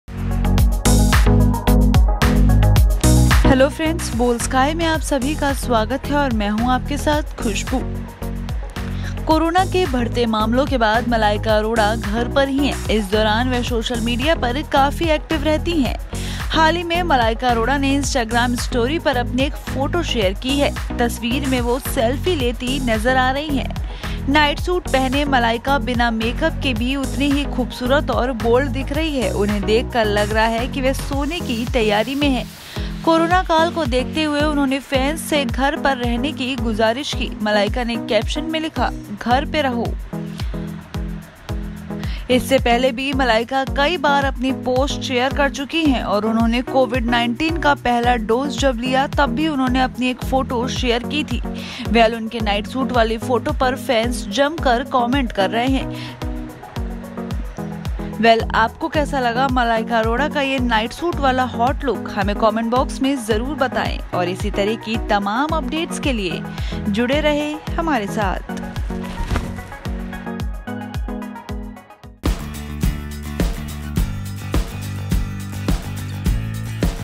Malaika Arora का Black Nightsuit में HOT LOOK VIRAL | Boldsky
कोरोना के बढ़ते मामलों के बाद मलाइका अरोड़ा घर पर ही हैं। इस दौरान वह सोशल मीडिया पर काफी सक्रिय हैं और लगातार पोस्ट कर रही हैं। मलाइका ने इंस्टाग्राम स्टोरी पर अपनी तस्वीर साझा की। तस्वीर में वो सेल्फी ले रही हैं। नाइटसूट पहने मलाइका बिना मेकअप के भी उतनी ही खूबसूरत दिख रही हैं। उन्हें देखकर लग रहा कि वो सोने की तैयारी में हैं।